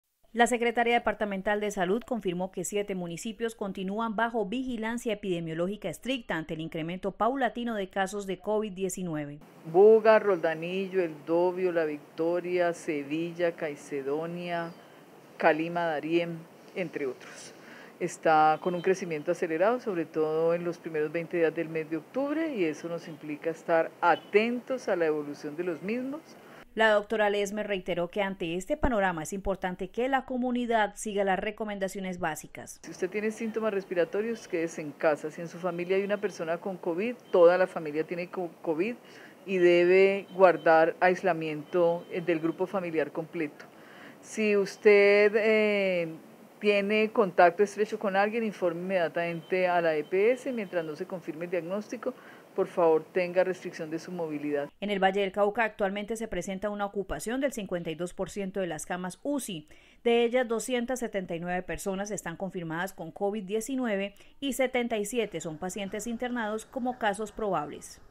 Bajo vigilancia epidemiológica siete municipios del Valle, por aumento de casos de Covis-19 DIA
Emitido el 22  de  octubre 2020 por el Canal regional Telepacífico.